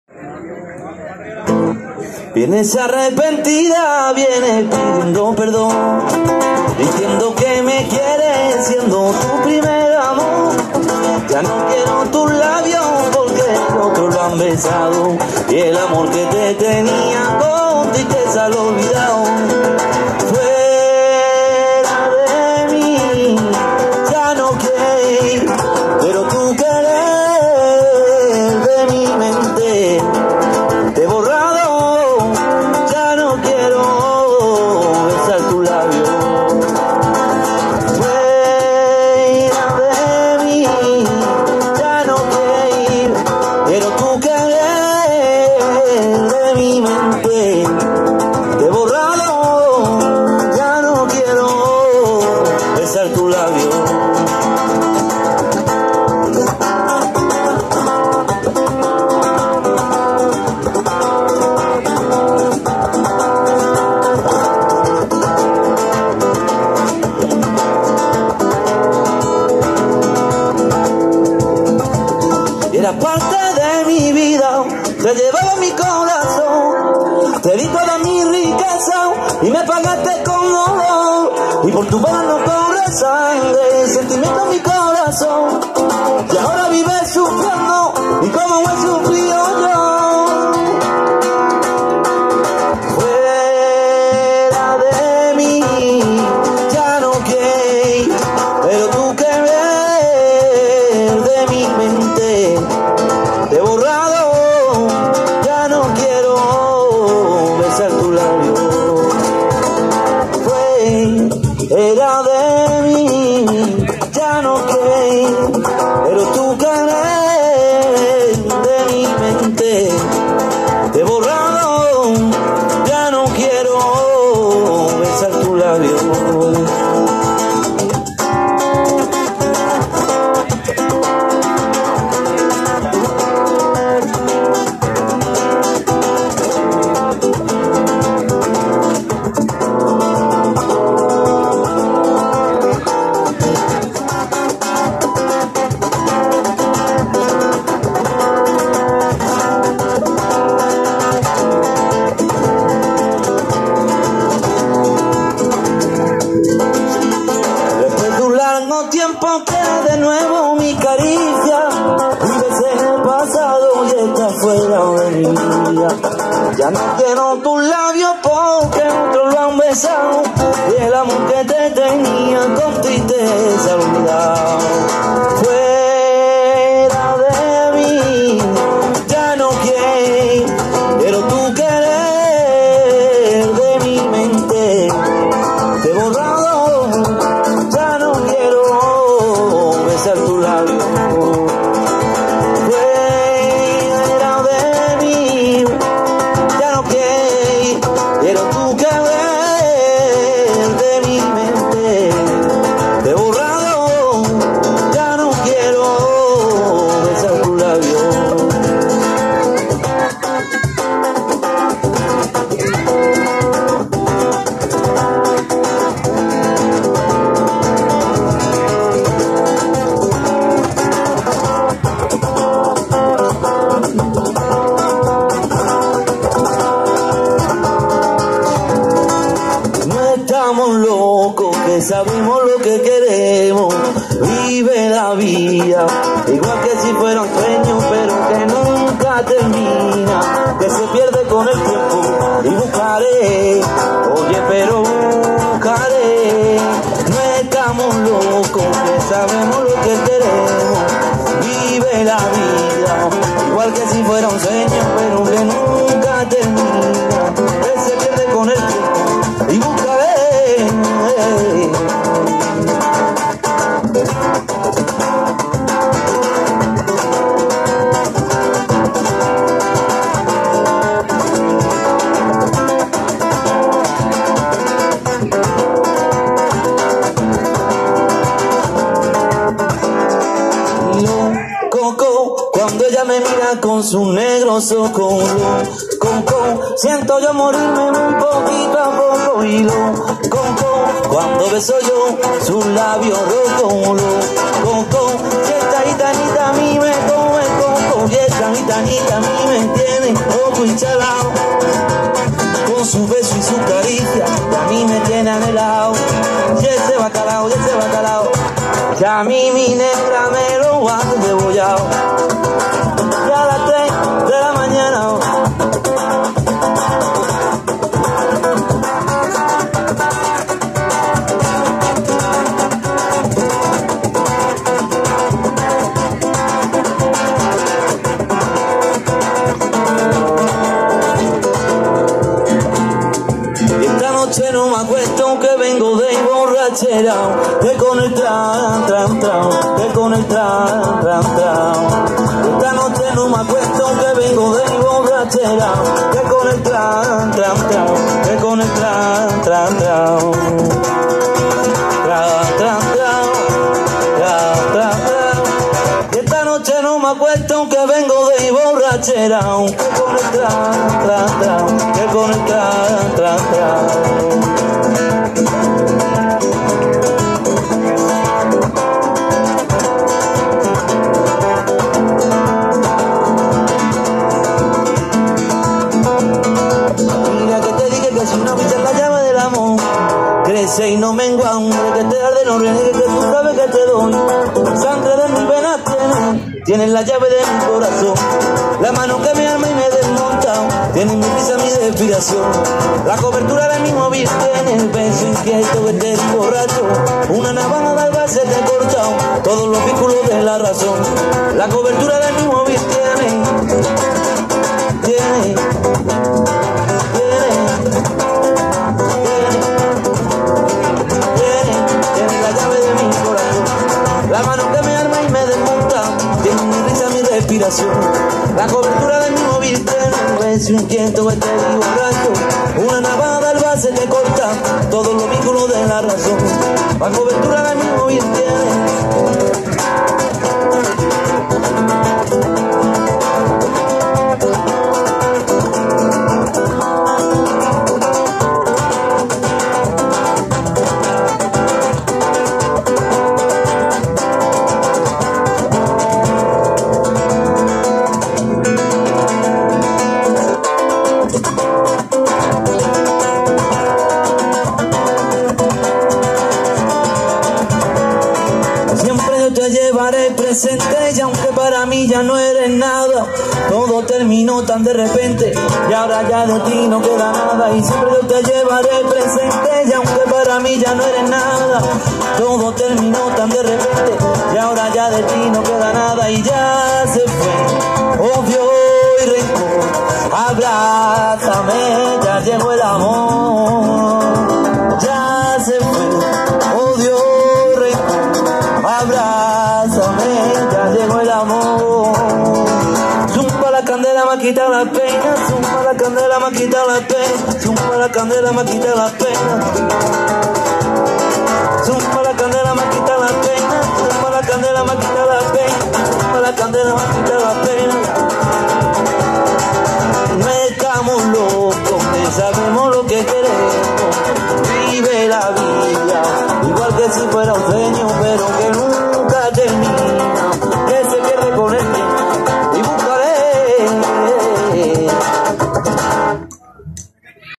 Sintonía Flamenca (1)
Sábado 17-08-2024
Concentración coches clásicos
Ugena (Toledo)